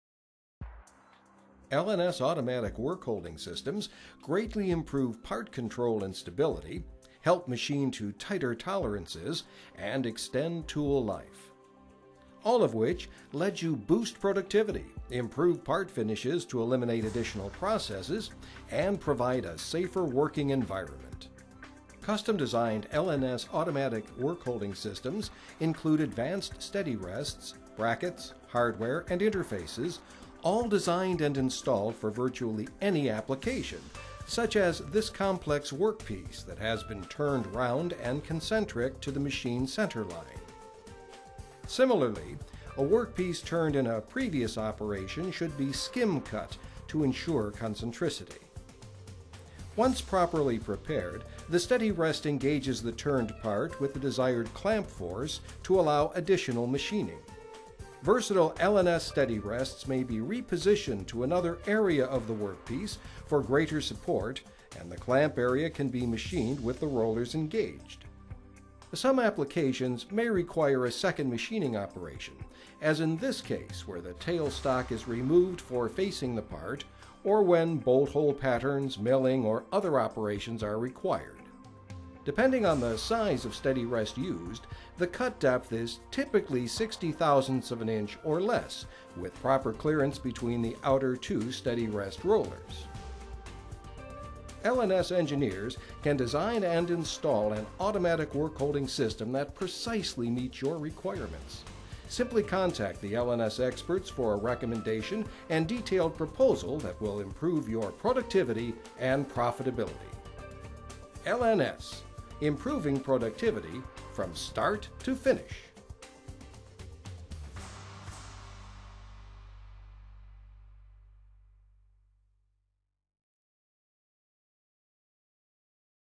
Steady Rest
LNS America custom designed high-performance, automatic work support systems by combining best-in-class steady rests with unmatched application knowledge, design engineering and exacting manufacturing.